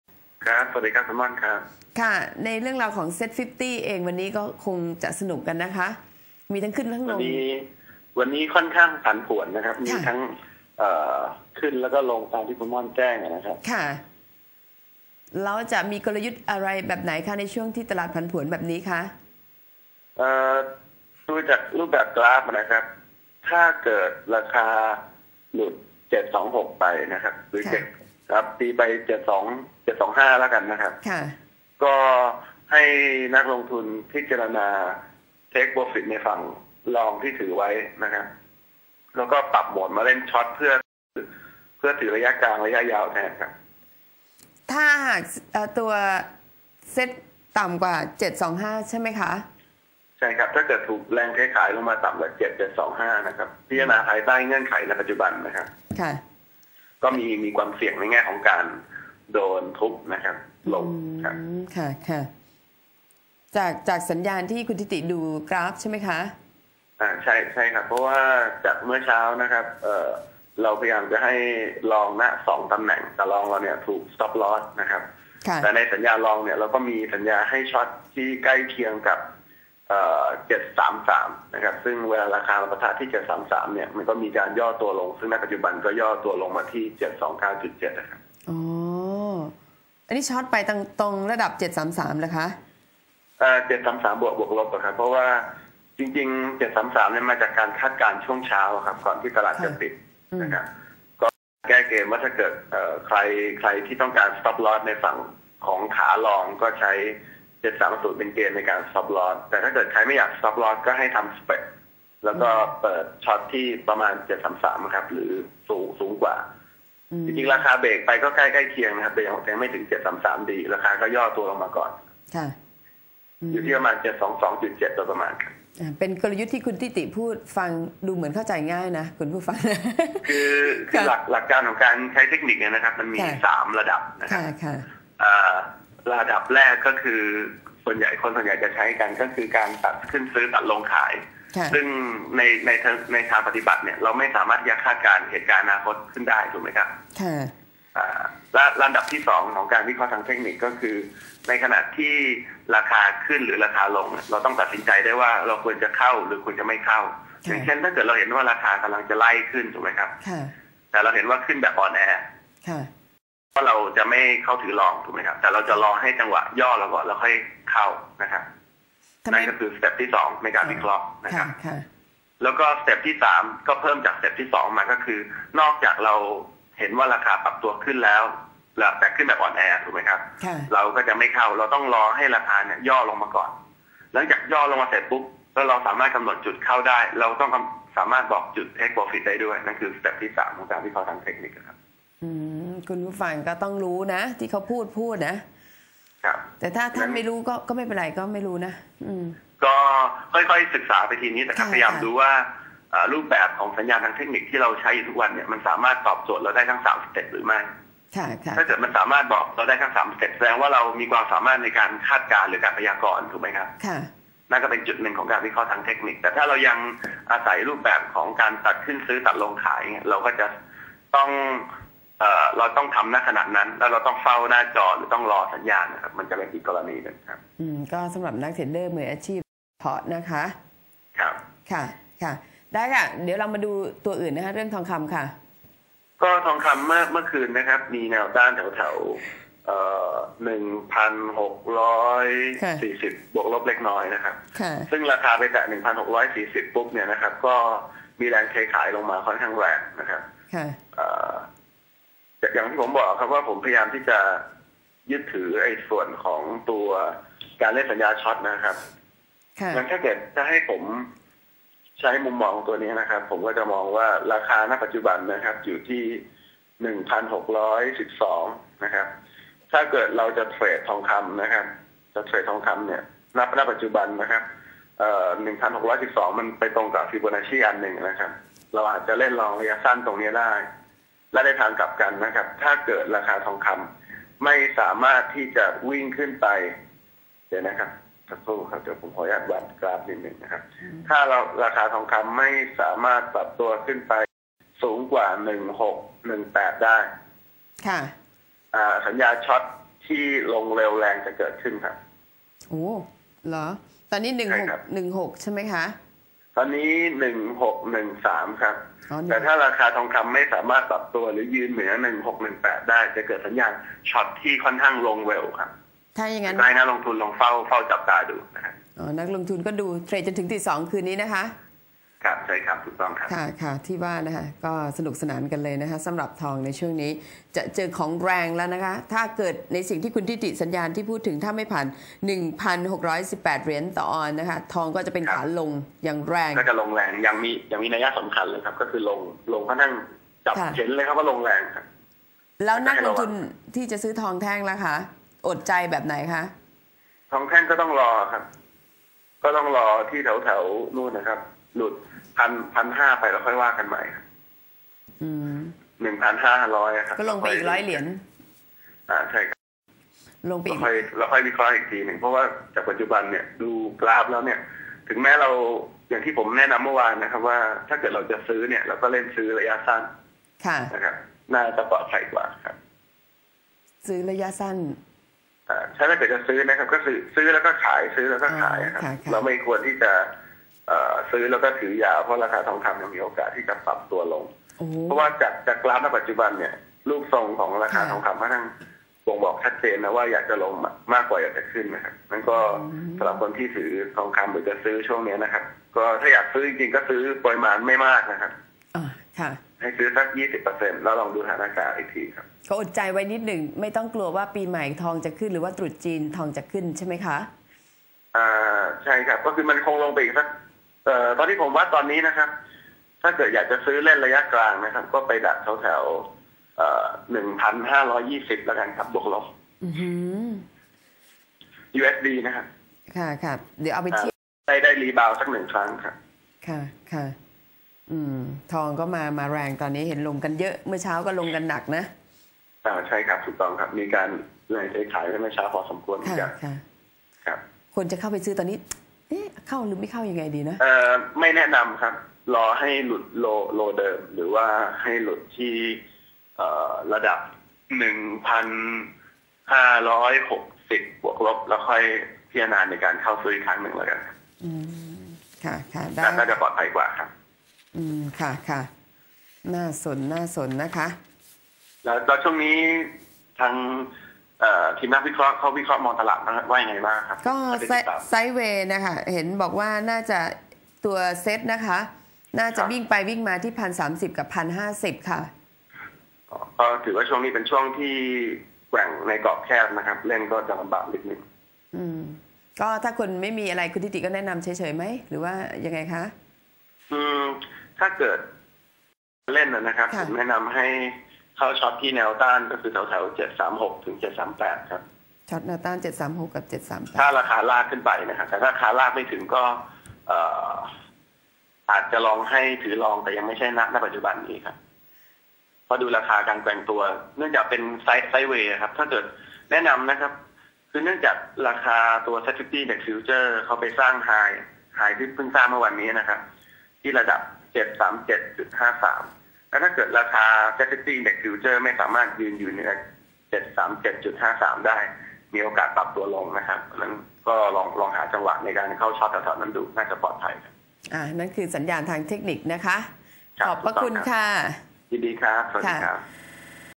ธิติ ธาราสุข 22-12-54 On Business Line & Life
ประธานเจ้าหน้าที่บริหาร บริษัท ชาร์ตมาสเตอร์ จำกัด เรื่องเทคนิคการลงทุน Set50 Short 733